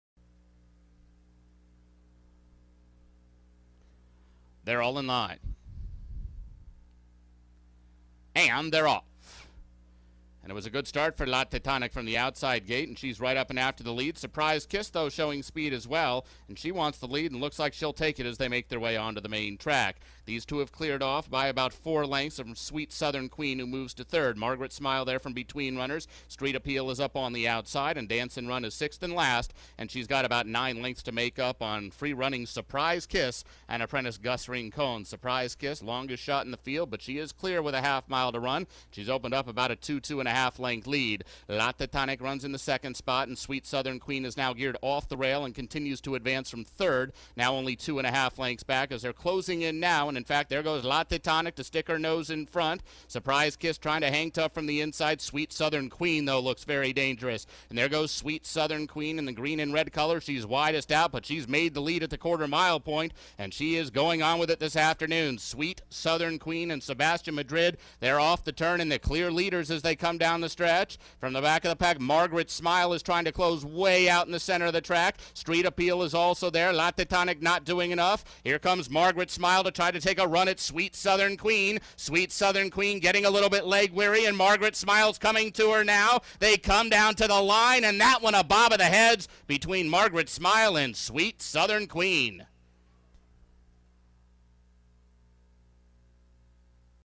CALDER RACE COURSE, 2009-10-24, Race 4
Entries: Thoroughbred Race, CALDER RACE COURSE, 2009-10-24, Race 4:
      (Pgm-Runner-Jockey-Finish)
       1-Surprise Kiss (FL), Rincon, G, finished 6;
       2-Dance N Run (FL), Aguilar, M, finished 5;
       3-Sweetsouthernqueen (FL), Madrid, S, finished 2;
       4-Margaret Smile (FL), Lezcano, A, finished 1;
       5-Street Appeal (KY), Olivero, C, finished 3;
       6-La Tetanic (FL), Garcia, L, finished 4;